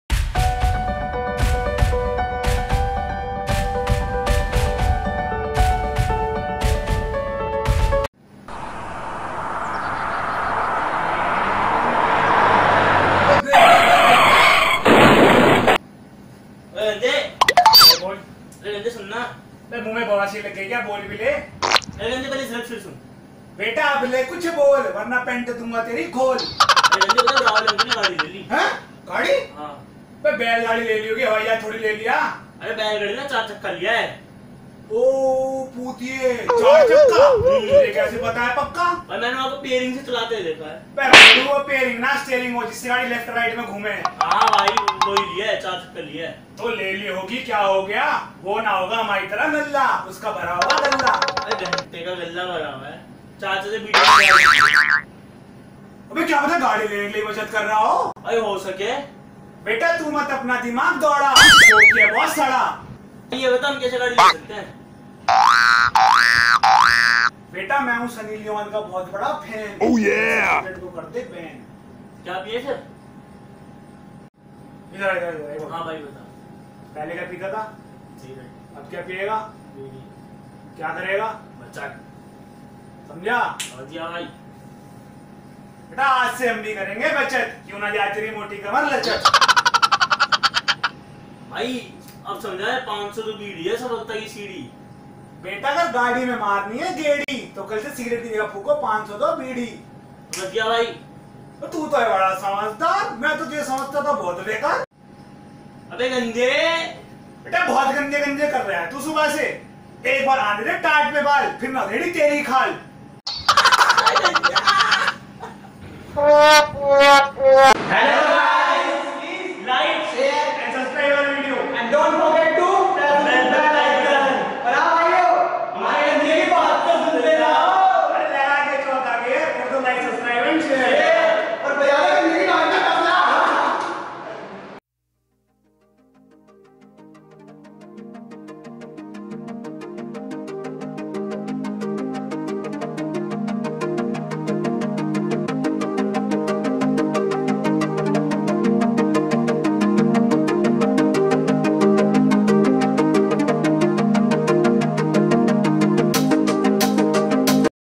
Bachat he hai Safalta ki punji "Firstyou"
"Firstyou" presents:- A video having lots off fun in it.. your eyes will fill with tears due to laughter...

script is writen by my school friend vikash pandey
and all idea of this video is came from vikash pandey
thankyou.